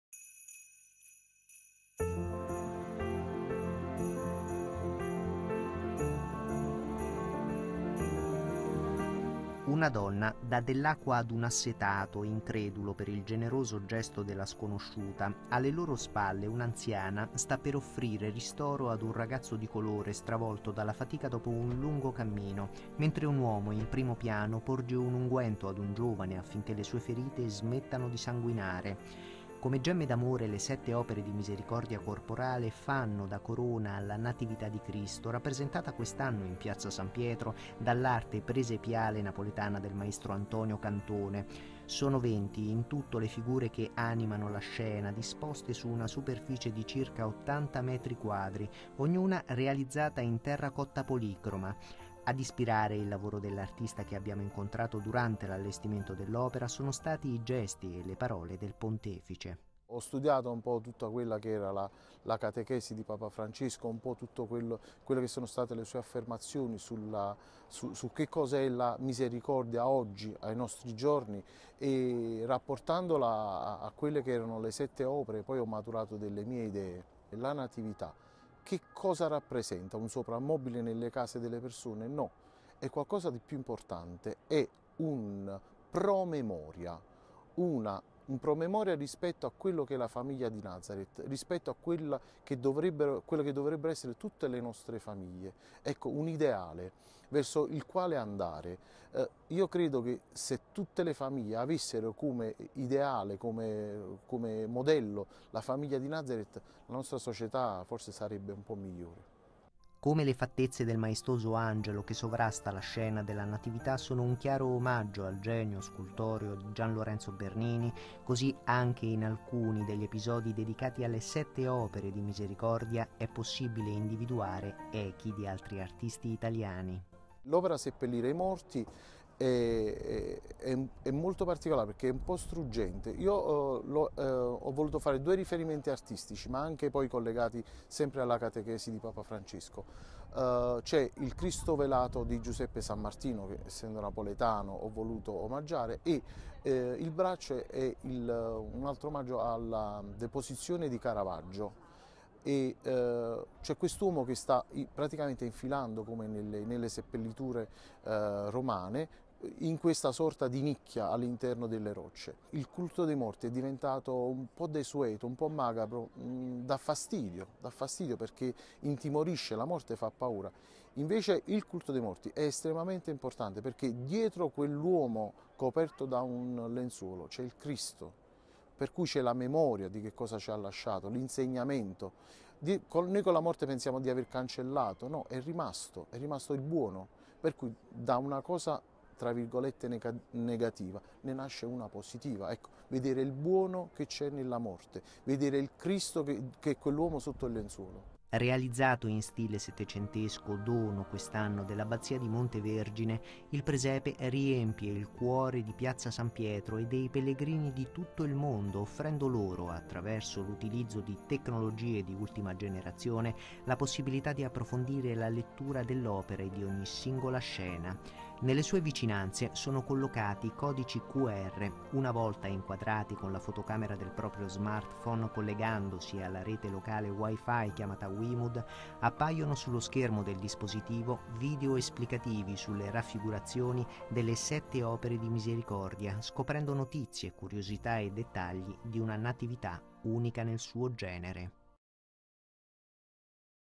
Vatican Magazine, "La Misericordia nel cuore di San Pietro", 07-12-2017
La Misericordia nel cuore di San Pietro

Ammiriamo il Presepe del Papa appena inaugurato in Vaticano, con 20 grandi figure in perfetto stile presepiale del settecento napoletano. Il maestro Cantone: ho studiato tutte le catechesi di Francesco sulla Misericordia, per rappresentare le sette opere.